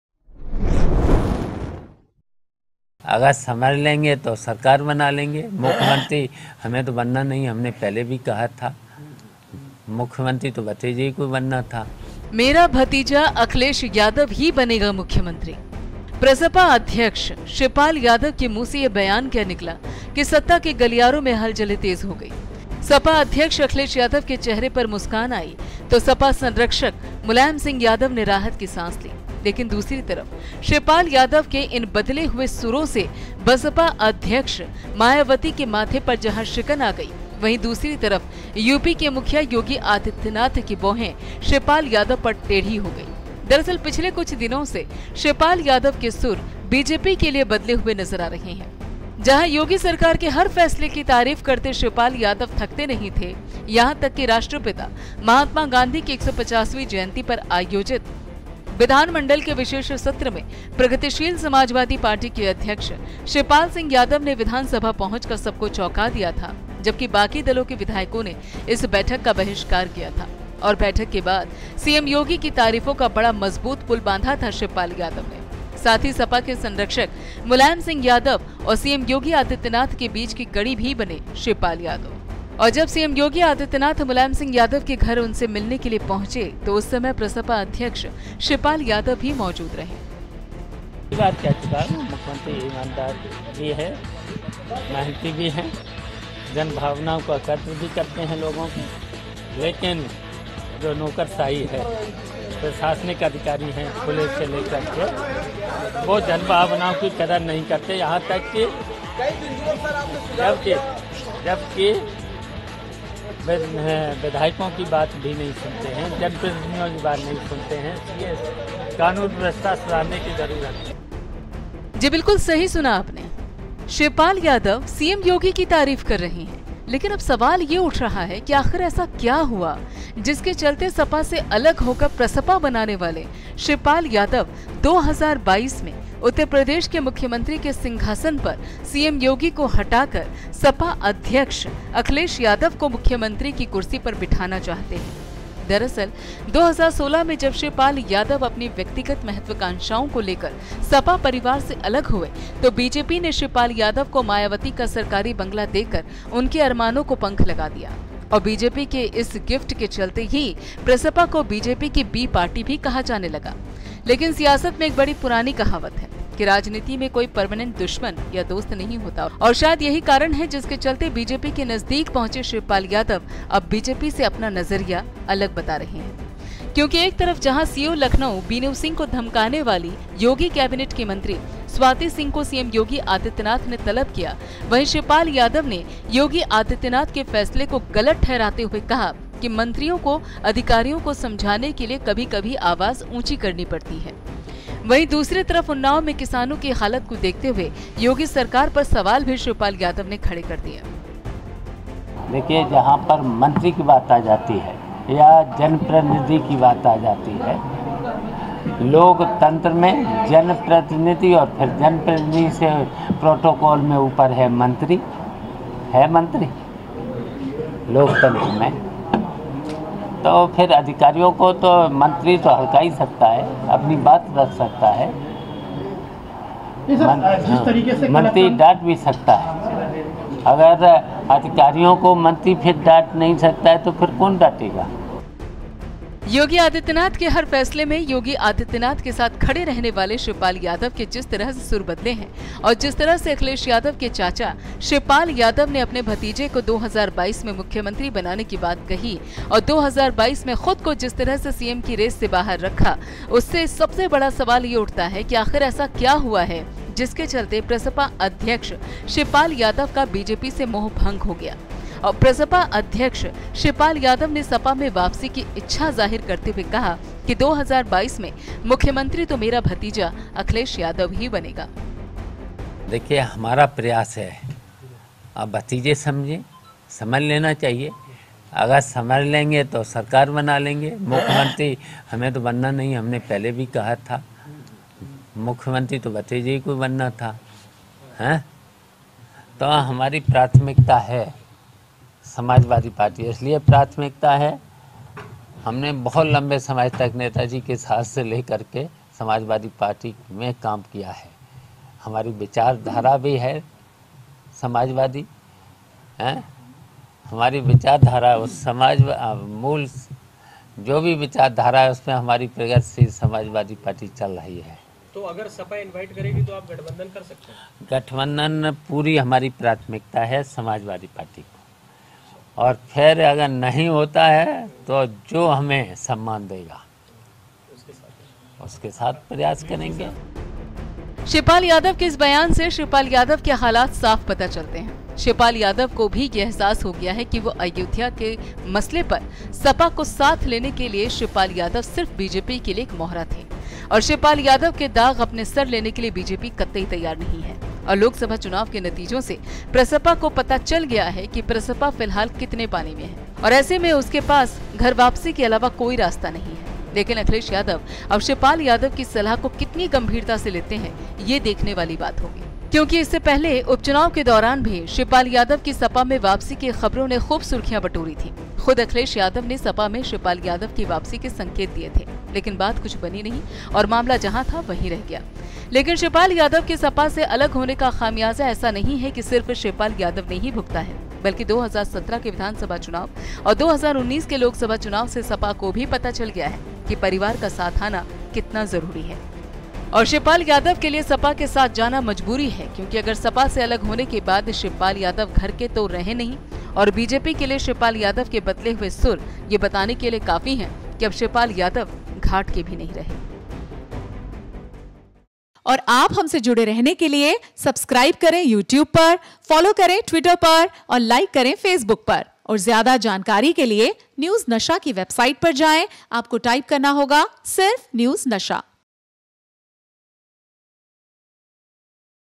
2022 में अखिलेश यादव होगें मुख्यमंत्री शिवपाल यादव | News nasha
मेरा भतीजा अखिलेश यादव ही बनेगा मुख्यमंत्री प्रसपा अध्यक्ष शिवपाल यादव के मुंह से ये बयान क्या निकला कि सत्ता के गलियारों में हलचलें तेज़ हो गई..सपा अध्यक्ष अखिलेश यादव के चेहरे पर मुस्कान आई..तो सपा संरक्षक मुलायम सिंह यादव ने राहत की सांस .